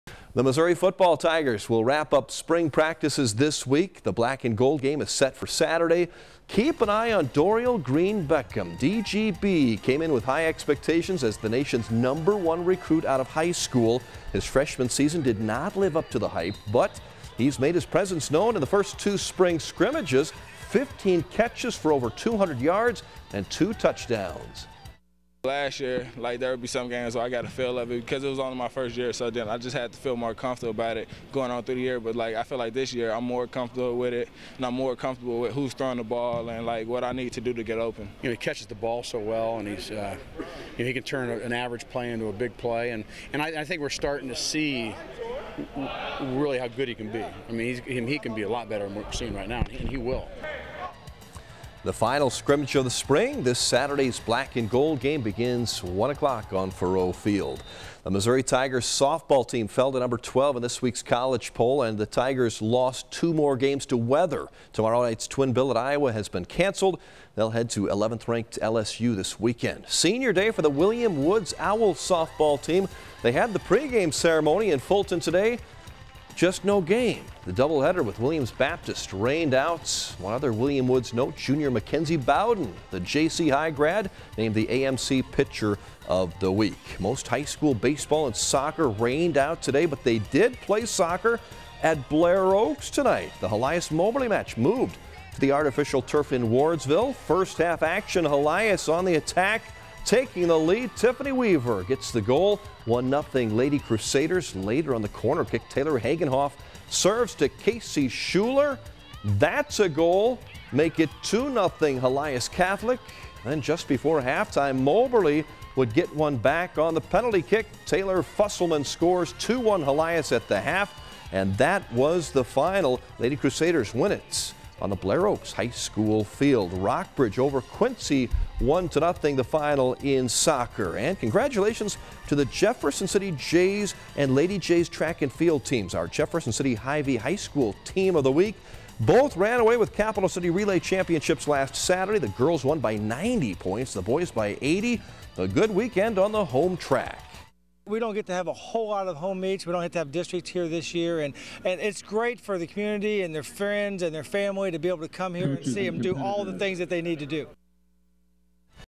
Dorial Green-Beckham living up to the hype this spring - April 16, 2013 - 10pm Sports
KRCG 13